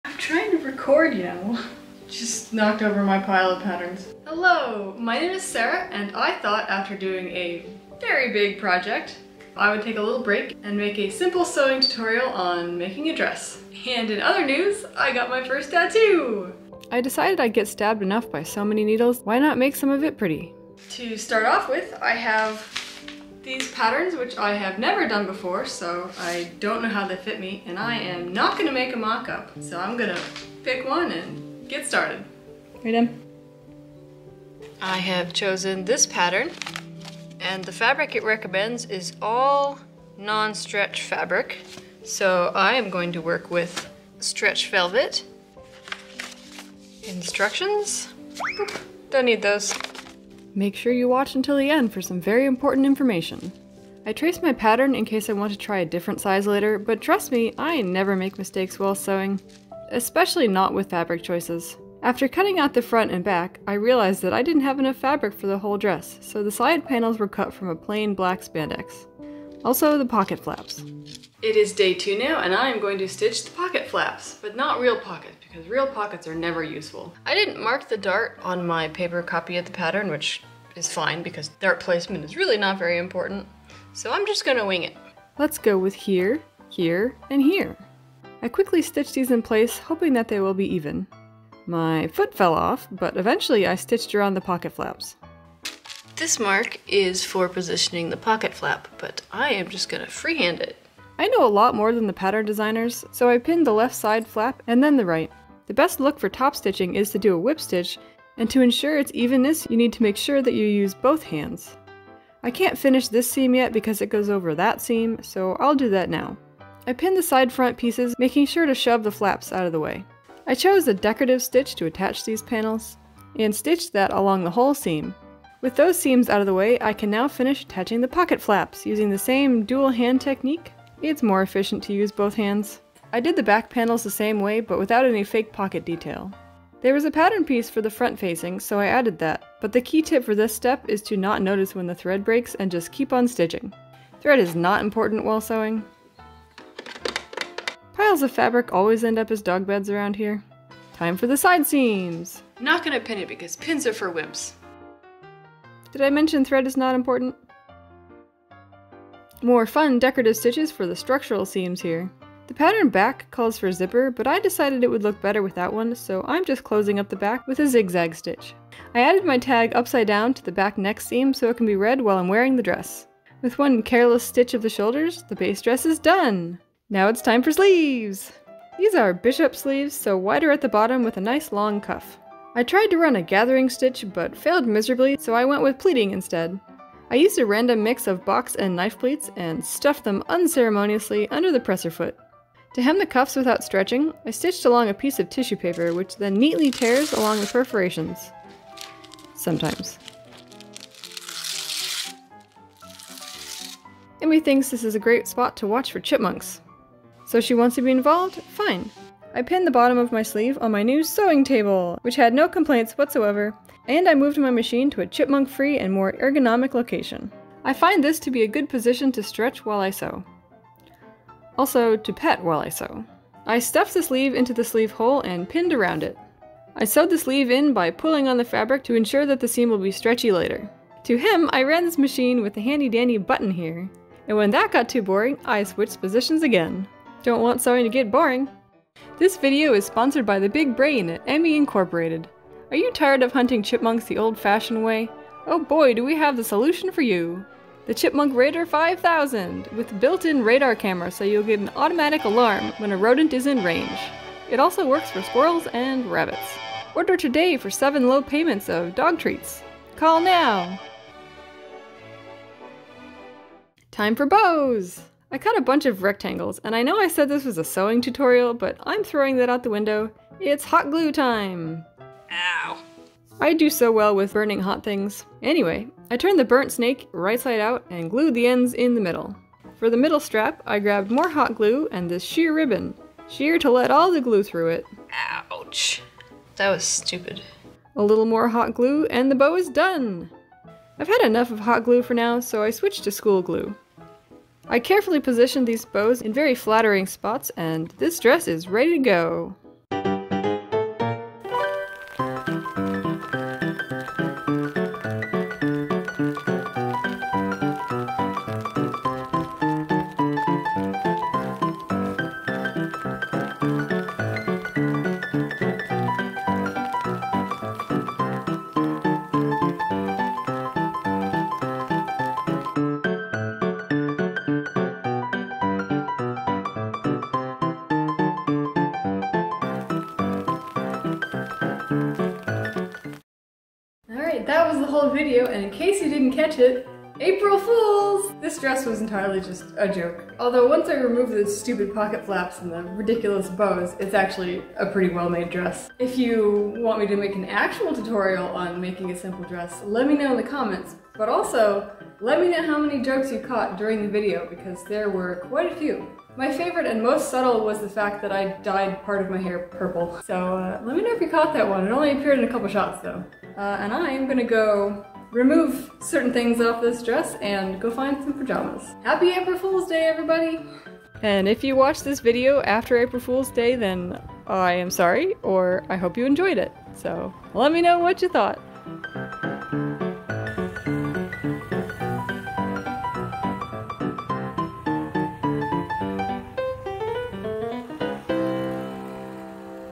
Simple dress sewing tutorial… Foolproof Dress DIY project for the 1st of April
Welcome to my first actual how to sew tutorial! I hope you like it, I tried to make it fool proof, but let me know what you think. I used my best sewing techniques and tips to help anyone from a beginner to an expert sewist to make a better garment.

Did you notice anything odd about some of the steps? Maybe something to raise an eyebrow at? Remember, it’s all in good fun—after all, it’s April Fools’ Day!

Chapters:
00:00 Intriduction
01:11 Chaos
02:12 more chaos
03:25 ummm, what??
03:58 Sponsor...?
04:23 WHo needs sewing anyway
04:59 what is going on???
05:34 Ooh that explains it